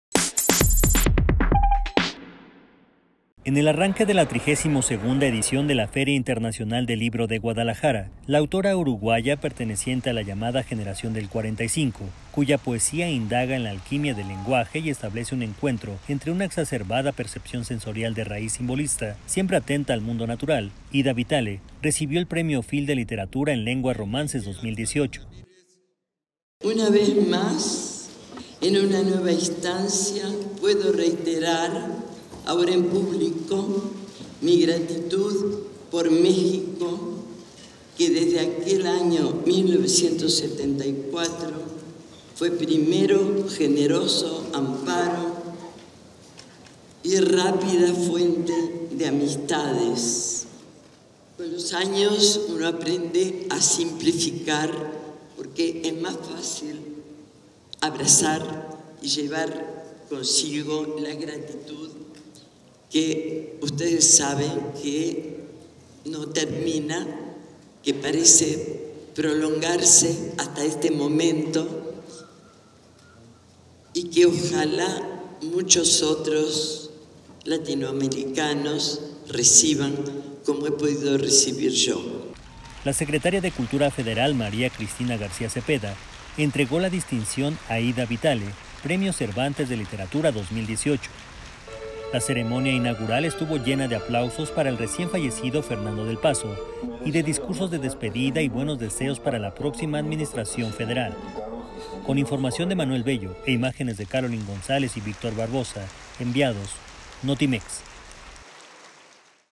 FIL 2018 arranca con premio de Literatura en Lenguas Romances a la poeta Ida Vitale
La secretaria de Cultura federal, María Cristina García Cepeda, entregó la distinción a Ida Vitale, Premio Cervantes de Literatura 2018.